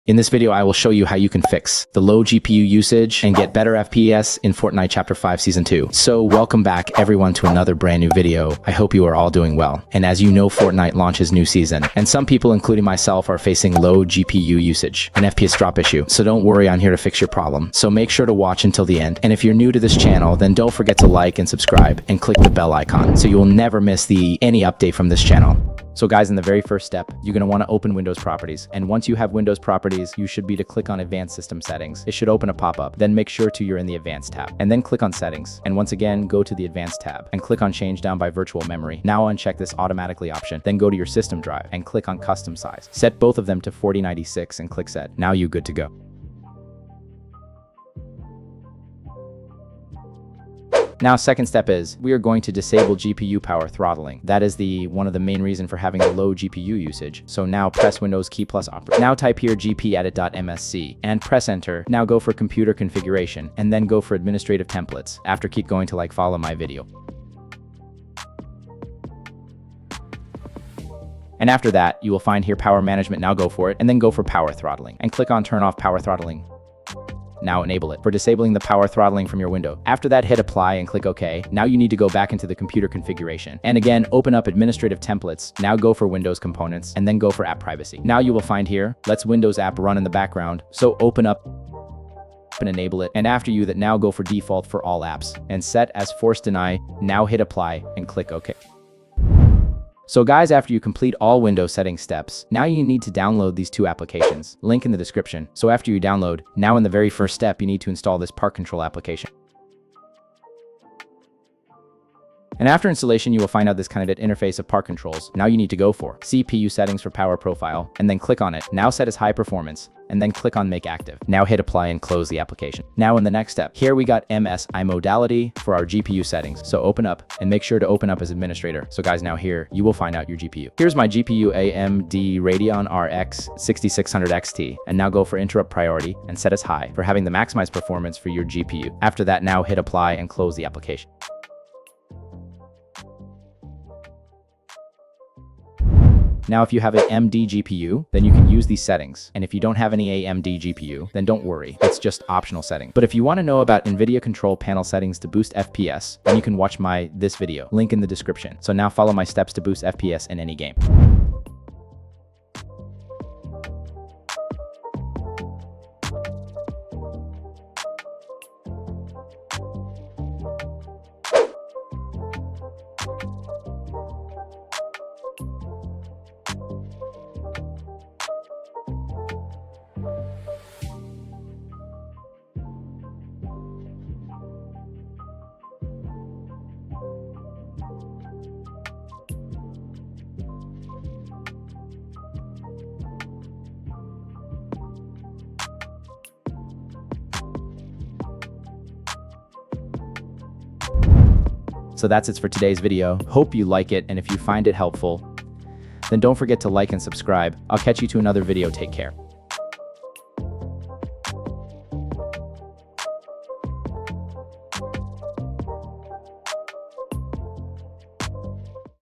How To Fix Low Gpu Usage In Fortnite Chapter 5 Season 2 ✅ - Get More FPS - Fortnite Season 2
Title : How To Fix Low Gpu Usage In Fortnite Chapter 5 Season 2 ✅ - Get More FPS - Fortnite Season 2

Videos Chapters :
00:00 intro
00:25 Virtual Memory
00:51 Power Throttling
1:40 Optimization Pack
1:47 Park Control
2:02 Msi Mode Utility
2:26 AMD Settings
3:21 Ending

PC Spec's
Processor : Ryzen 5 5600
Motherboard : Msi B450m A-Pro Max
Gpu : XFX Rx 6600xt
Ram : Delta T-Force 16gb
PSU : Corsair TX750